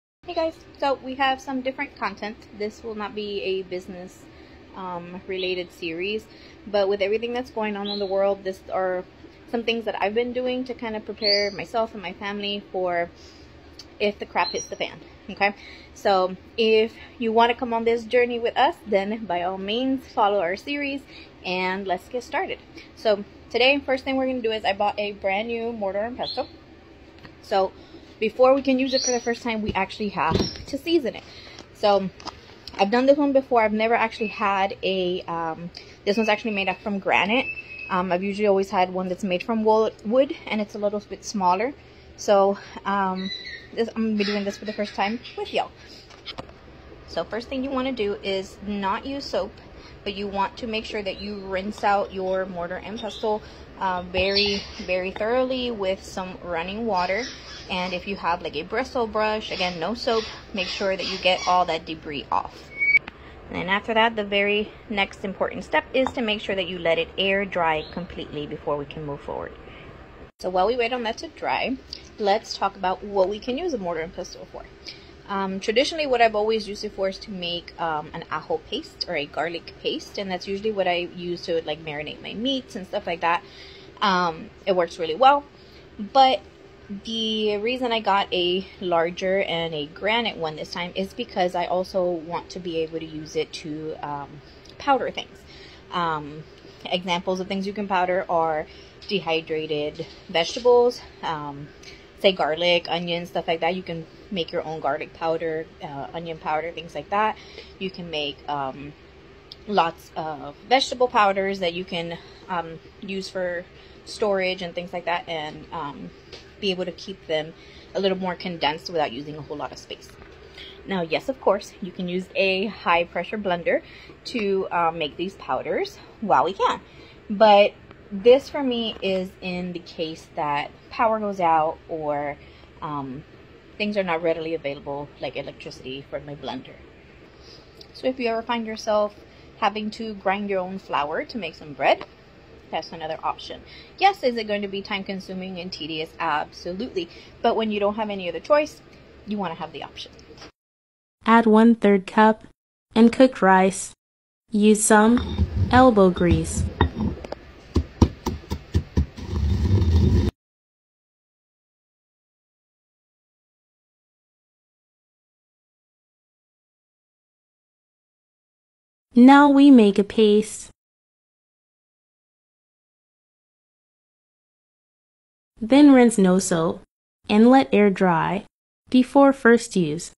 Season a Mortar & Pestle with me
Seasoning a brand new mortar and pestle

Link to this Mortar & Pestle